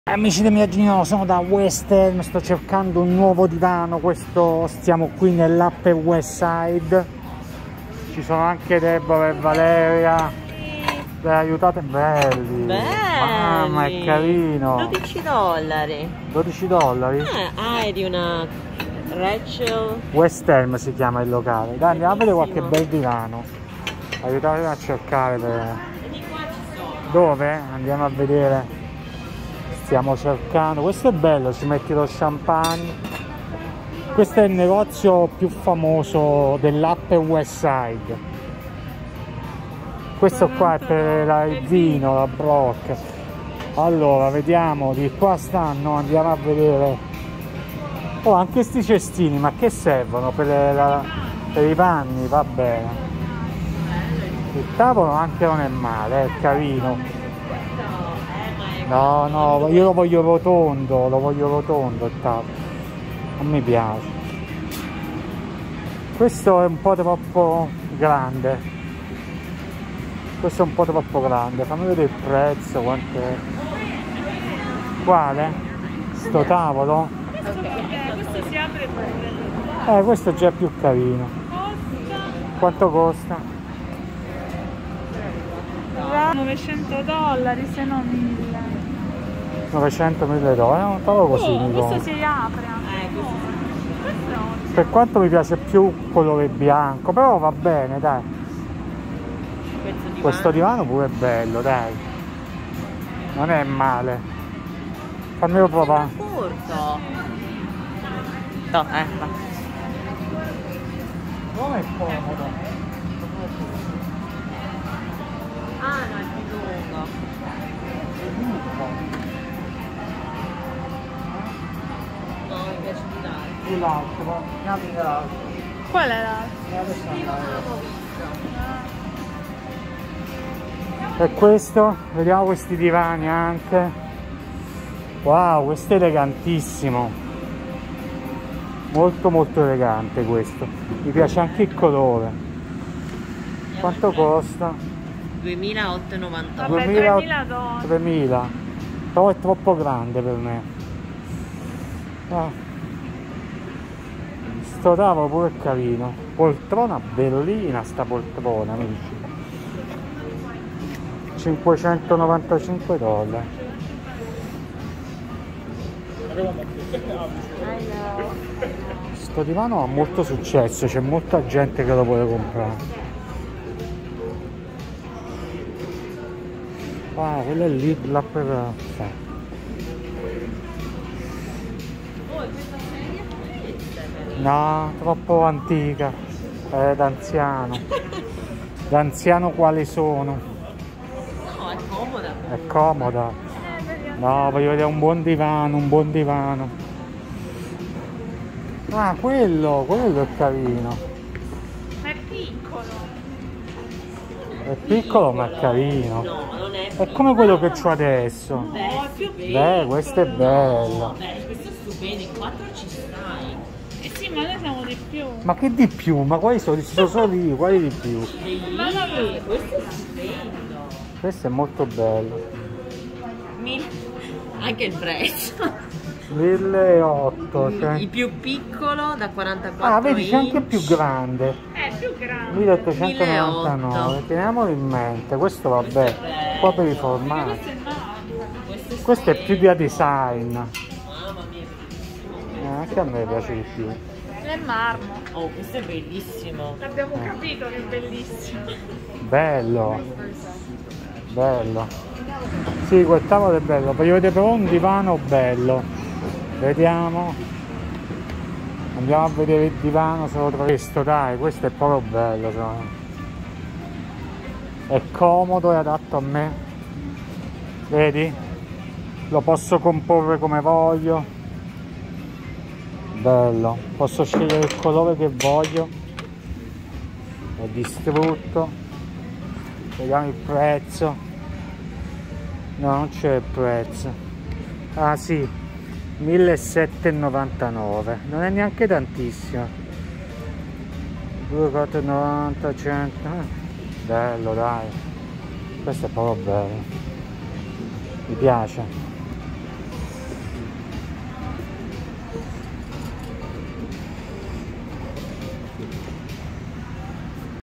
Negozio d'arredamento dell'Upper West Side, West Elm
Amici del mio viaggio a New York, eccomi da West Elm, uno dei negozi di arredamento piu' belli dell'Upper West Side. Stavo cercando un divano, ma alla fine mi piace molto un tavolo. Consigliatemi.

👉 Il Romanzo:

Vanity Fair